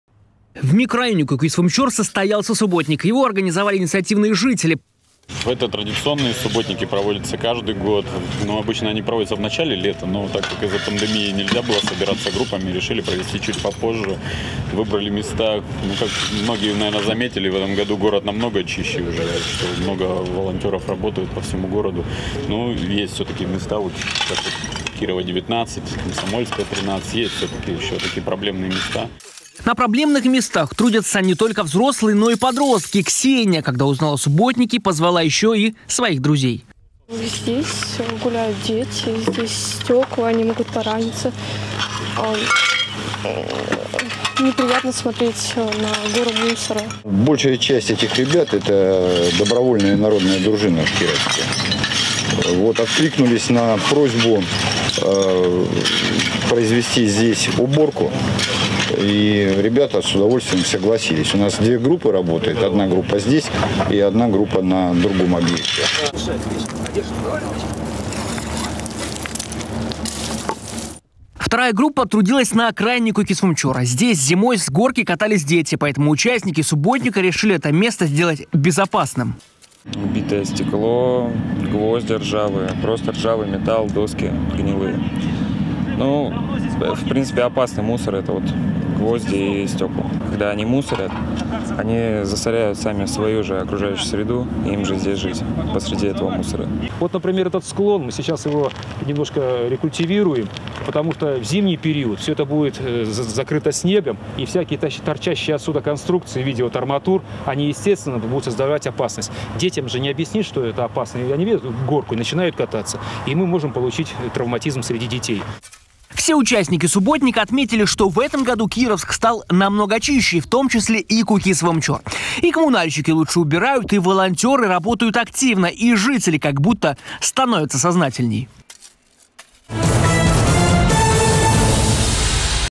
В микрорайоне Кукисвумчорр прошел традиционный субботник
В микрорайоне Кукисвумчорр прошел традиционный субботник. На проблемных местах трудились не только взрослые, но и подростки. Поддержал идею и председатель профкома Кировского рудника Евгений Рассказов.